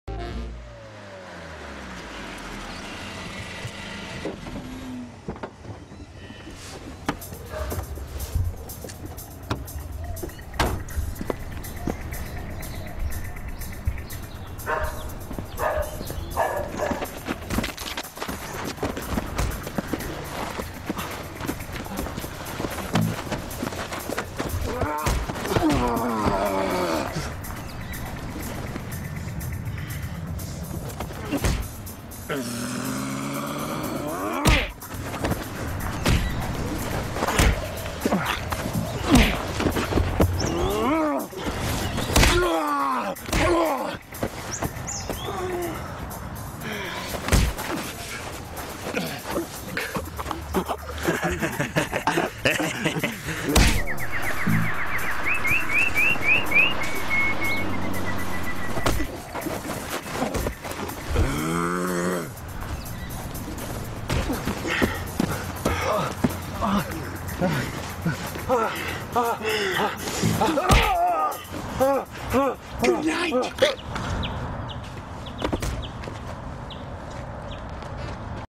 :The John Trader Archives:
Scenes of a film that I canned, A hitmans  been called to "take care" of John Trader but sometimes things dont turn out as planned.